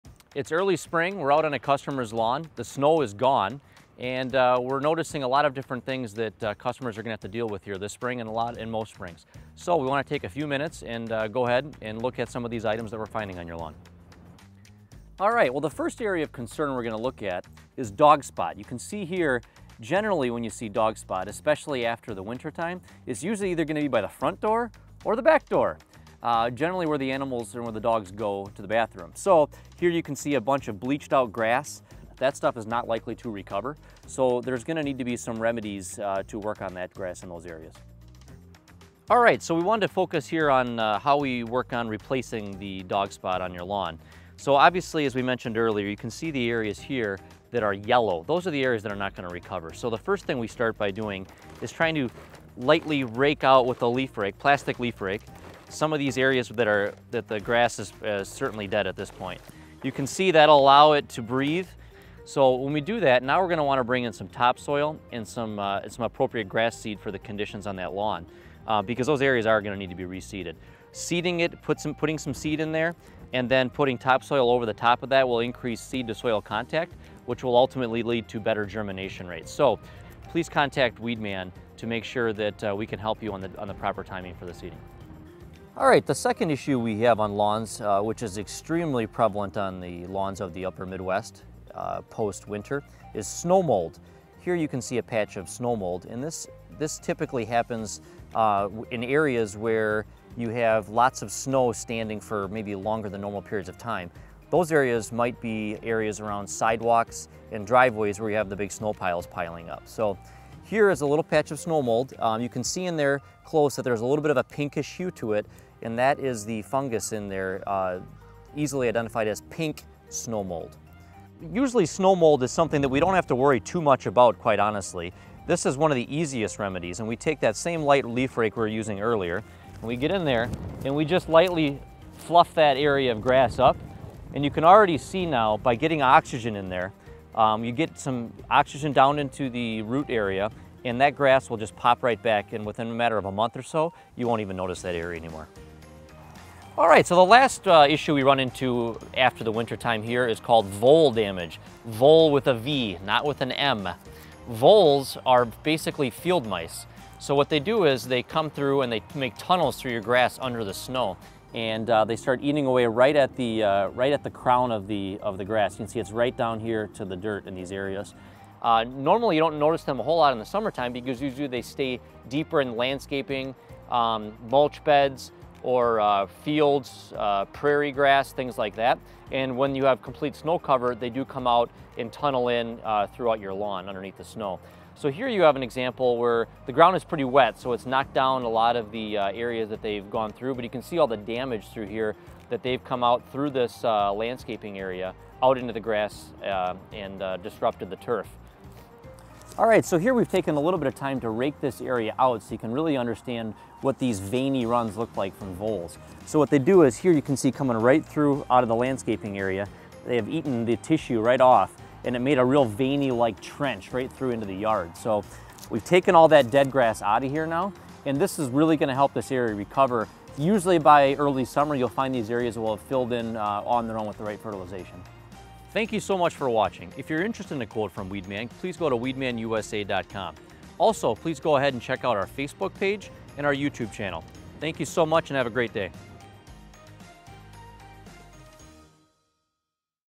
Dog Spot, Snow Mold, & Vole Damage
Andy Kurth walks through common spring issues on a customer's lawn, and offers simple remedies to help get your lawn back in shape for summer.